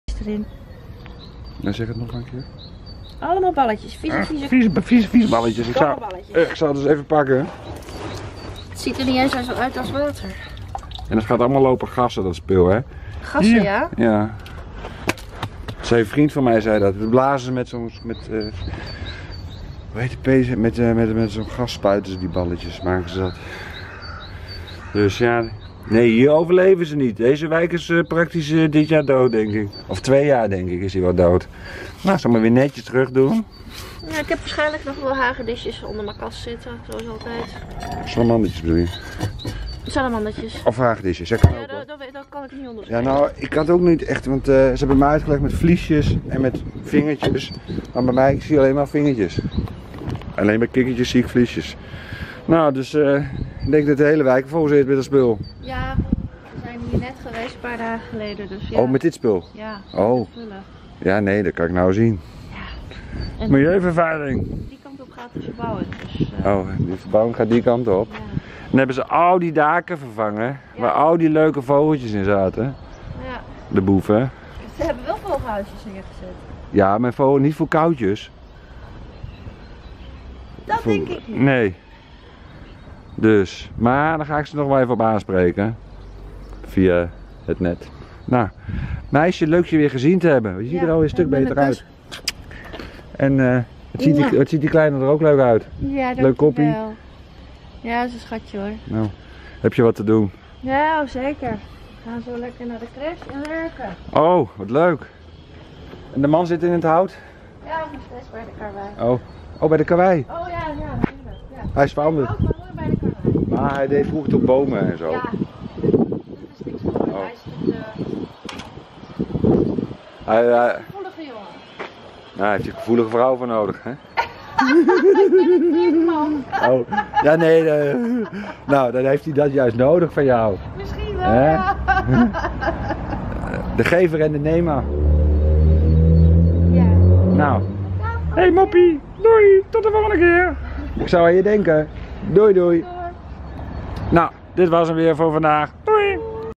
De hele wijk vol balletjes. Alle daken zijn energie neutraal gemaakt en kauwtjesvrij. 2018.. ECOSIDE
De woningbouw heeft wel een paar mussenhuisjes opgehangen. En totaal niks voor de kauwtjes gedaan. Terwijl het nachtbroeders zijn. Volgens de wet mag de woningbouw elke keer als ze niet zitten te broeden. Het nestje dichtmaken. Ze hoeven geen nestkasten op te hangen voor de kauwtjes. Vogelbescherming AntieFauna. Agenda 21 Label. Overal hebben dieren hulp nodig. Geef hun jouw Stem.  EU&VN@AntieFauna.